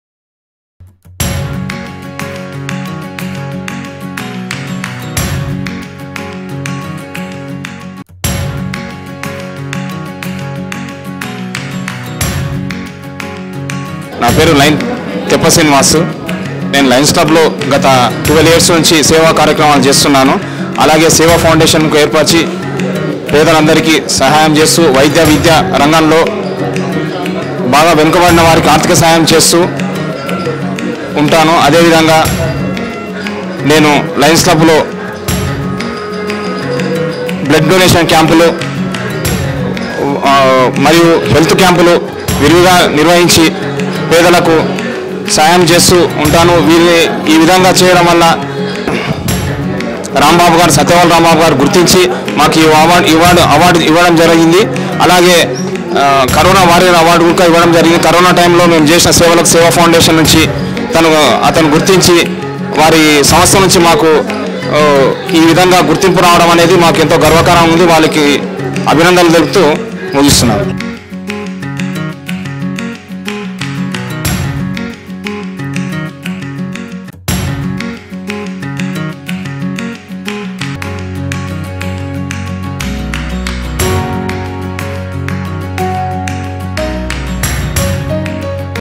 Sri Teppa Srinu, Social Activist, Nagarkurnool District, Telangana State, India.
Sri Teppa Srinu got Viswaguru World Records "Swamy Vivekananda Indian Icon Awards-2021" in the category of Social Service on January 31st in Bharat vikas parishad, Kukatpally, Hyderabad.